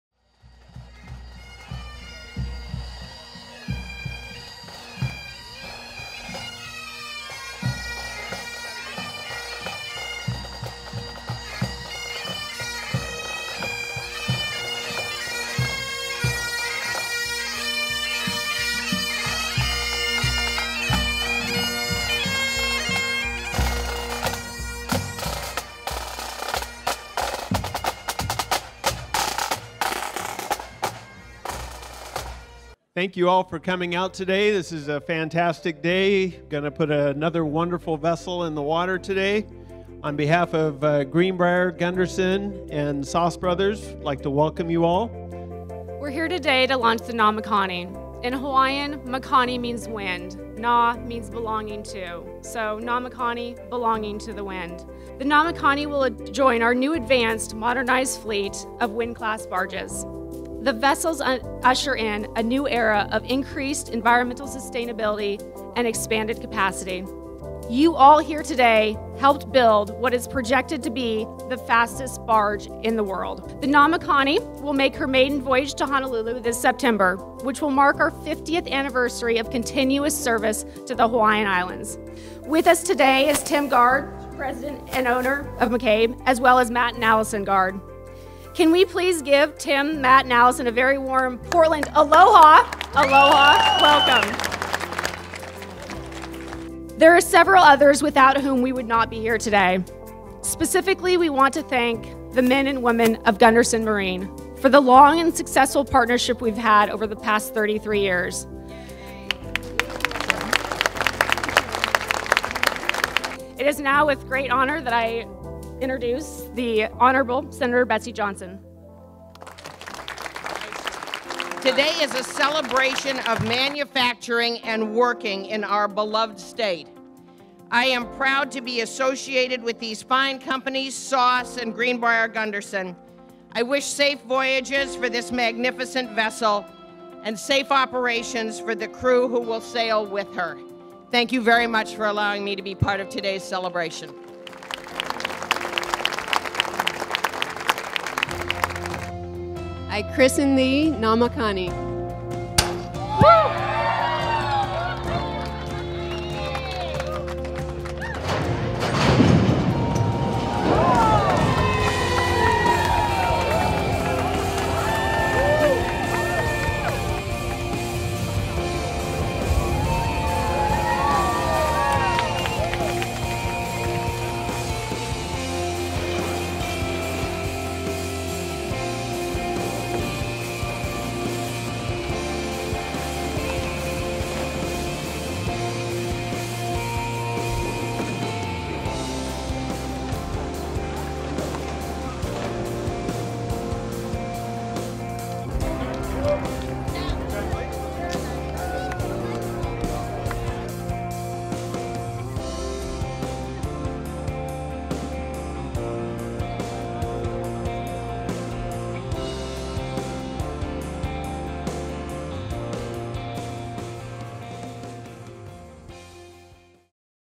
Gunderson Marine launches Heavy Deck Barge, NAMAKANI
Gunderson Marine launched Heavy Deck Barge, NAMAKANI for Sause Bros. on Saturday, April 30, 2016. A large crowd came out to see Sponsor, Allison Guard christen the hull and send the barge down the launch ways and into the Willamette River. The NAMAKANI will join the fleet of Sause barges working in the Hawaiian Islands.
NAMAKANI
Length: 438’
Beam: 105’
Depth: 25’
Designed by: Hockema & Whalen Associates